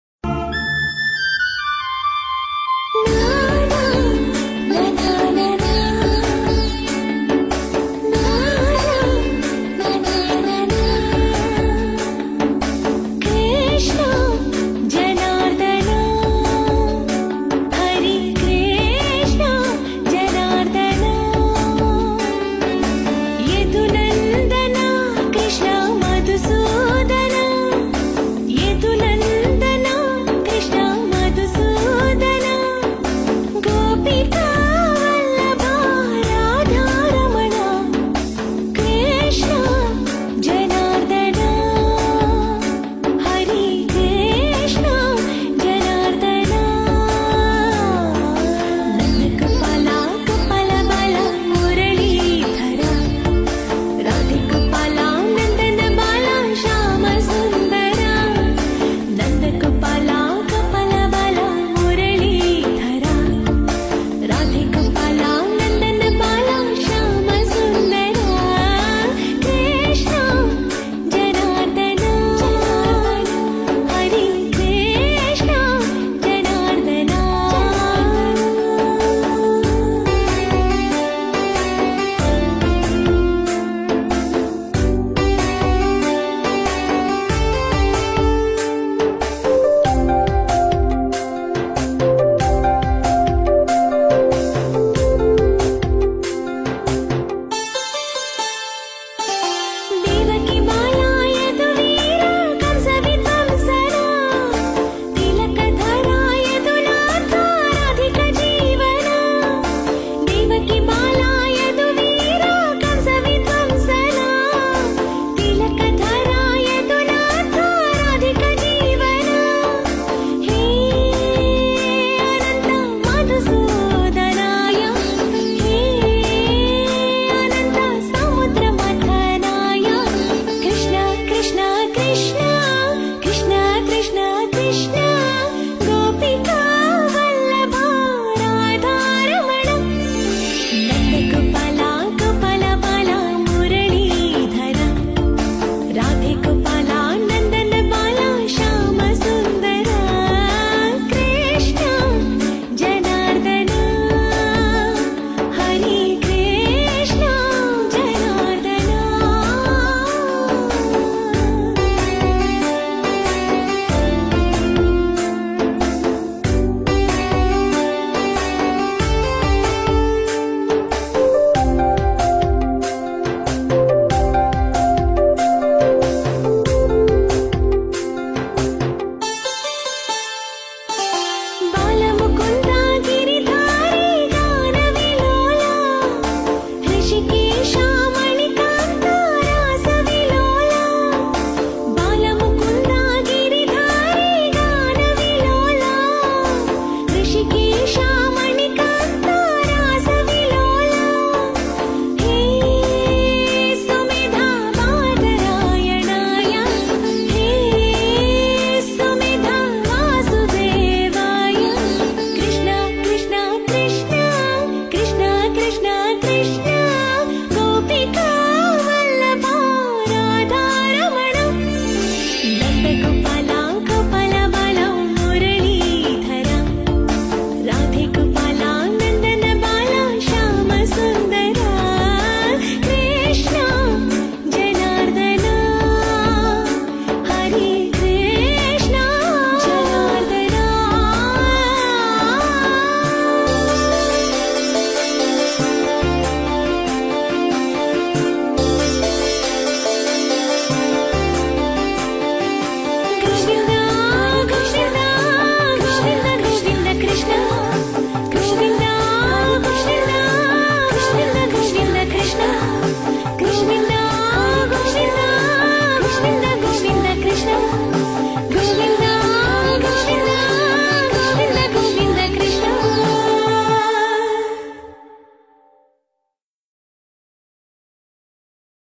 આ ગીત ની ખાસ વાત એ છે. કે શ્રીકૃષ્ણ ના ૧૦૦/ નામ સીવાય બીજો કોઈ શબ્દ આવતો નથી. પરવેઝ મુસ્તુફા.....
આ ગીત ની ખાસ વાત એ છે. કે શ્રીકૃષ્ણ ના ૧૦૦/ નામ સીવાય બીજો કોઈ શબ્દ આવતો નથી. અને ગાયિકા છે પ્રસિધ્ધ ગાયક મહંમદ રફી ની દિકરી. પરવેઝ મુસ્તુફા.